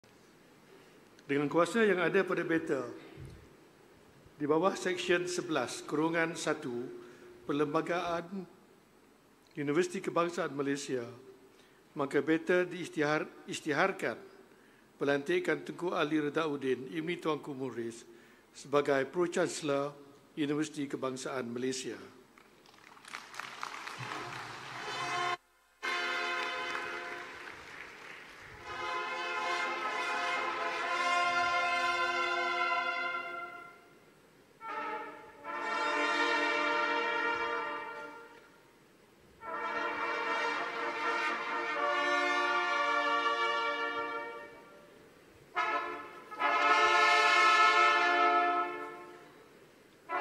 PERISYTIHARAN PELANTIKAN PRO-CANSELOR UKM - YANG AMAT MULIA TUNKU BESAR SERI MENANTI TUNKU ALI
Perisytiharan Pelantikan Pro-Canselor Yang Amat Mulia Tunku Besar Seri Menanti Tunku Ali Redhauddin Ibni Tuanku Muhriz sebagai Pro-Canselor, Universiti Kebangsaan Malaysia